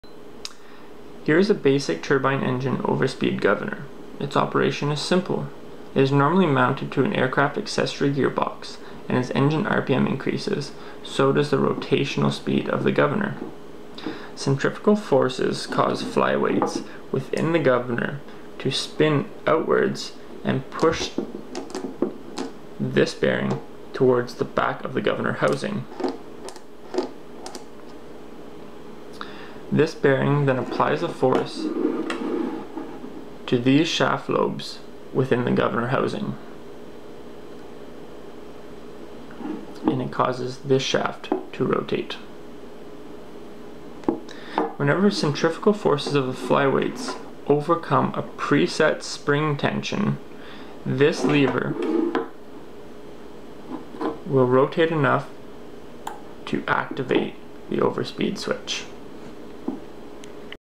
TURBINE ENGINE GOVERNOR
This video describes the very basic operation behind a turbine engine over-speed governor. It is out of a Solar T41 APU engine.